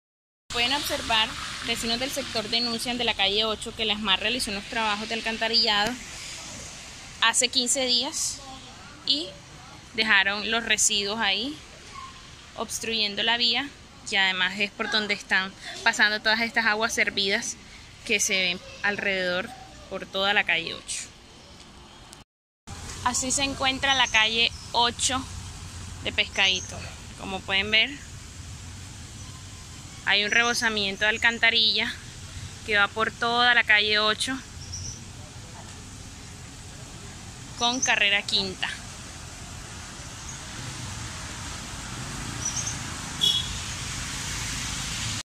Aguas servidas en calles de Pescaíto, problema de no acabar
Esta mañana los habitantes del barrio más popular de Santa Marta, Pescaíto, amanecieron con las calles inundadas, no sólo por las correntias de agua lluvia, sino por un derramamiento de aguas servidas.